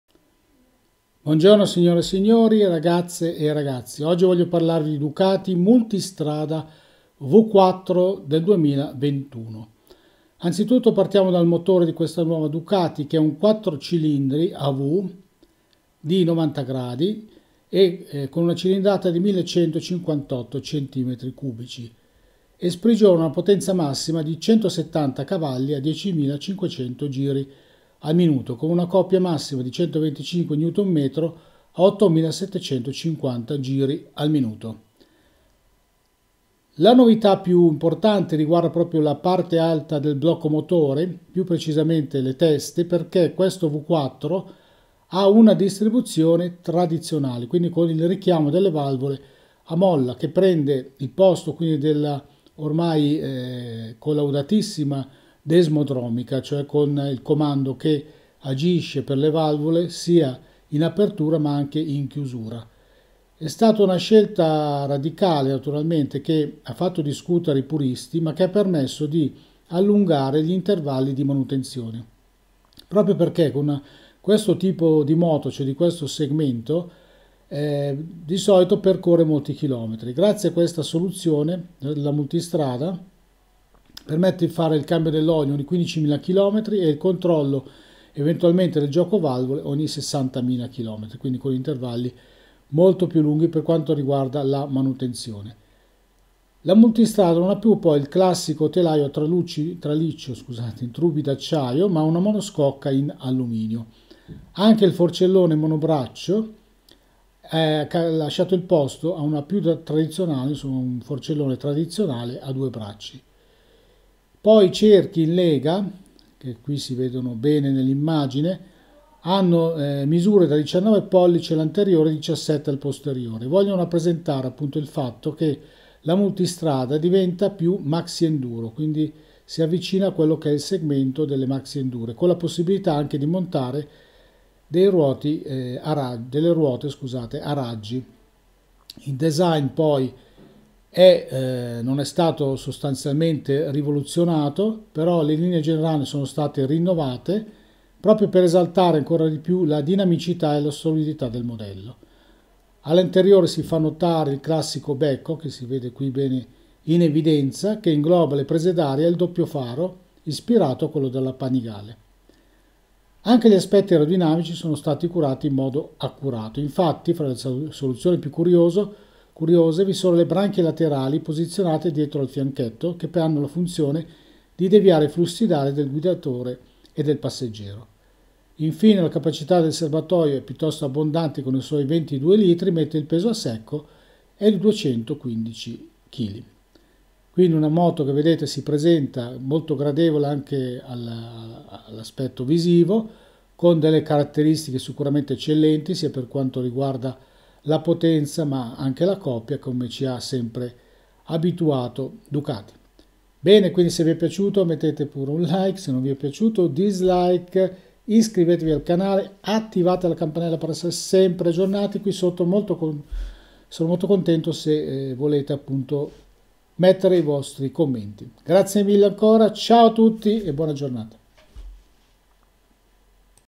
336 Ducati Multistrada V4 2021
Moto da turismo moderne

   Ducati Multistrada V4 2021

   Il motore di questa nuova Ducati è un quattro cilindri a V di 90° da 1.158 centimetri cubici con una potenza massima di 170 cavalli a 10.500 giri al minuto ed una coppia massima di 125 Nm a 8.750 giri al minuto.

   La novità riguarda la parte alta del blocco, più precisamente le teste: infatti, questo V4 ha la distribuzione tradizionale con il richiamo delle valvole a molla, che prende il posto della desmodromica, ormai un “must” sulle Ducati degli ultimi decenni.

   Una scelta radicale, che ha fatto discutere i puristi, ma che ha permesso di allungare gli intervalli di manutenzione. Una moto di questo segmento può percorrere molti chilometri e, grazie a questa soluzione tecnica, la Multistrada V4 permette di effettuare il cambio dell’olio ogni 15.000 chilometri ed il controllo ed eventuale regolazione del gioco valvole, ogni 60.000 km.

   La Multistrada non ha più il classico telaio a traliccio in tubi d’acciaio ma una monoscocca in alluminio. Anche il forcellone monobraccio per una soluzione più tradizionale a due bracci.

   I cerchi, in lega, con misure di 19” all’anteriore e 17” al posteriore vogliono rappresentare il fatto che la Multistrada V4 diventa un po’ più maxienduro con la possibilità di montare i cerchi a raggi.

   Il design della nuova Ducati Multistrada V4 è forse l’unico dettaglio che non è stato rivoluzionato. Le linee generali sono state rinnovate per esaltare ancora di più la dinamicità e la solidità del modello. All’anteriore si fanno notare il classico becco, che ingloba le prese d’aria ed il doppio faro, ispirato a quello della Panigale.

   Anche gli aspetti aerodinamici sono stati curati in modo accurato. Infatti, fra le soluzioni più curiose, vi sono le branchie laterali posizionate dietro al fianchetto, che hanno la funzione di deviare i flussi d’aria dal guidatore e dal passeggero.

  La capacità del serbatoio è abbondante con i suoi  22 litri mentre il peso a secco è di  215 chilogrammi.